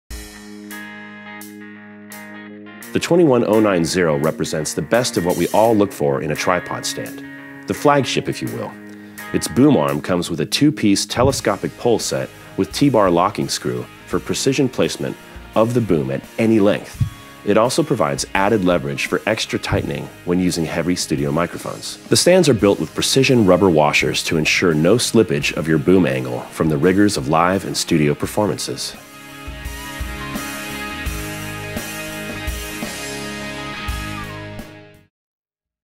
Tripod Mic Stand with Boom 21090
Review of the 21090 Tripod Mic Stand from König & Meyer. German Made. 5 Year Warranty. Eco-Friendly